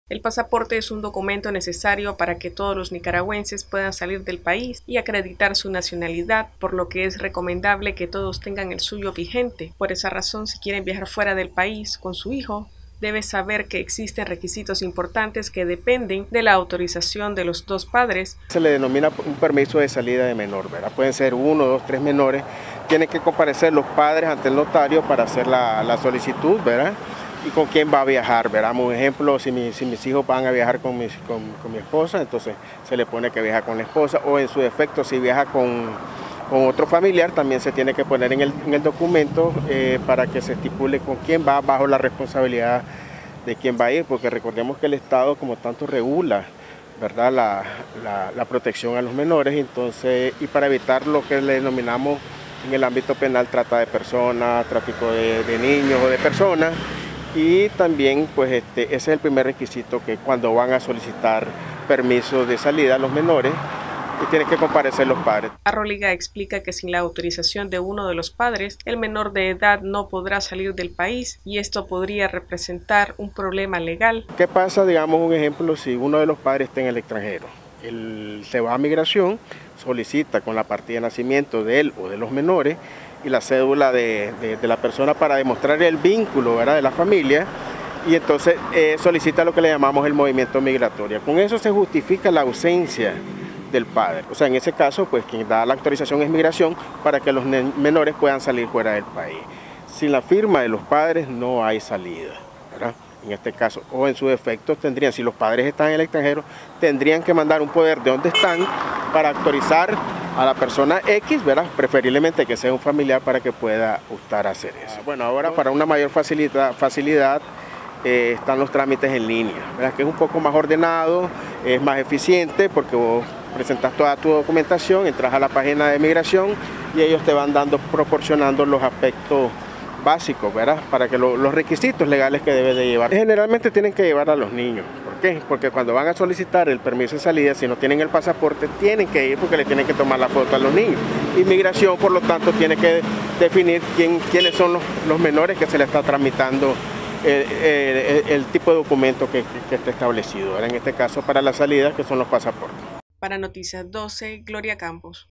Noticias12Nicaragua ¿Cuáles son los requisitos para sacar pasaporte de hijos menores de edad?
¿Cuáles son los requisitos para sacar pasaporte de hijos menores de edad?